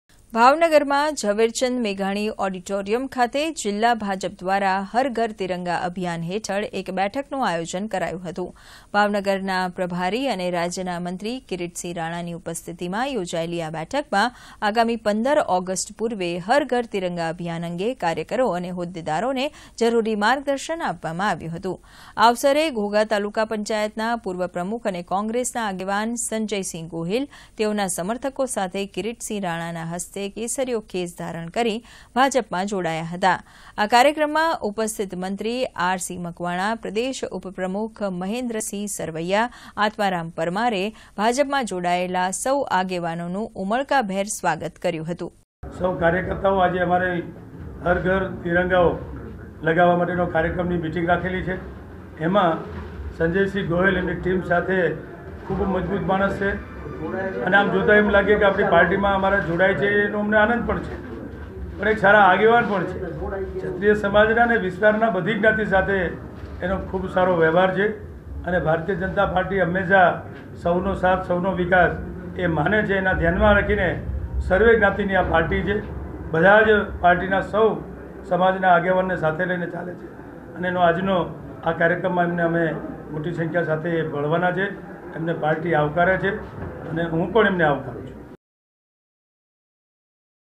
Bhavnagar જિલ્લા ભાજપની Har Ghar Tiranga અંતર્ગત બેઠક યોજાઈ
ભાવનગરમાં ઝવેરચંદ મેઘાણી ઓડીટોરીયમ ખાતે  જીલ્લા ભાજપ દ્વારા હર ઘર તિરંગા અભિયાન હેઠળ એક બેઠકનું આયોજન કર્યું.